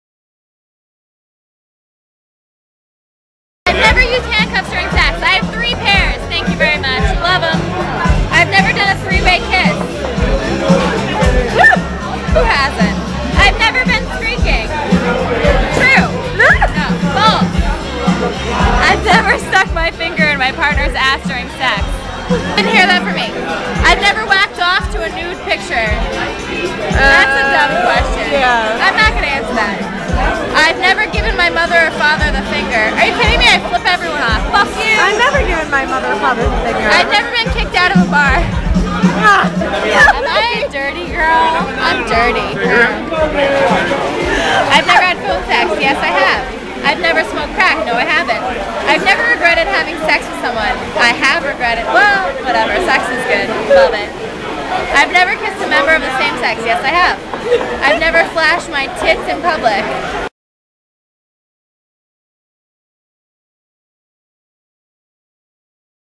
The "I've Never" Bar Cards
4 Margaritas + "I've Never" Bar Cards = truth serum

Kelly tells us all about her sex life & flashes everyone in the bar, all because of these "I've Never" Bar cards